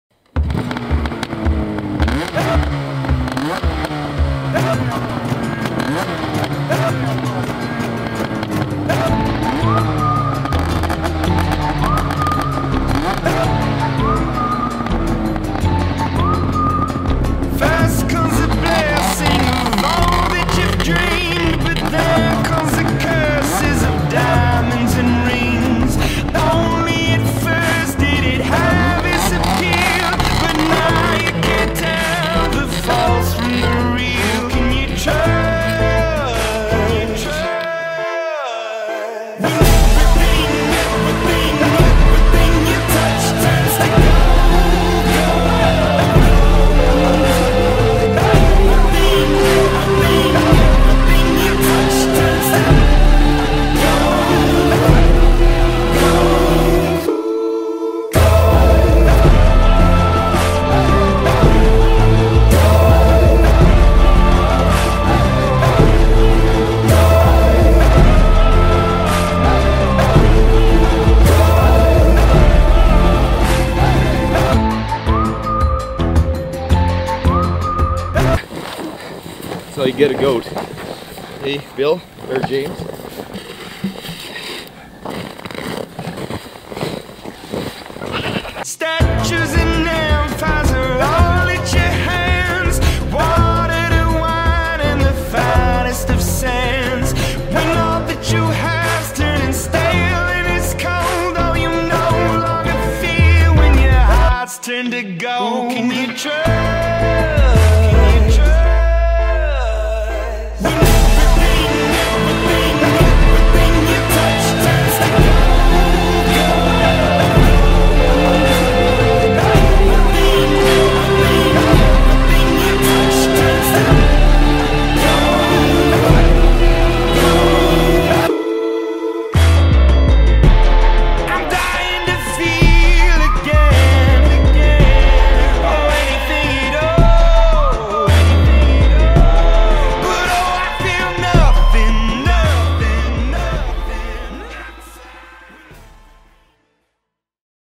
Josh Penner - Freestyle Snowmobile
Watch as Josh Penner trains for the 2017 X-Games.